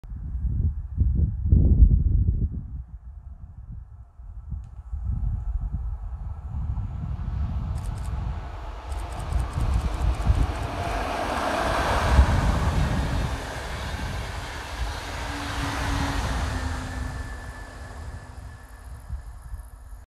D-DB 112 166 + D-TDRO 223 054-8 am Schluß mit DPF 300; Bln. Außenring zw. Karow West u. Schönfließ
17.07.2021
19.22 Uhr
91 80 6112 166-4 D-DB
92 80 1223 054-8 D-TDRO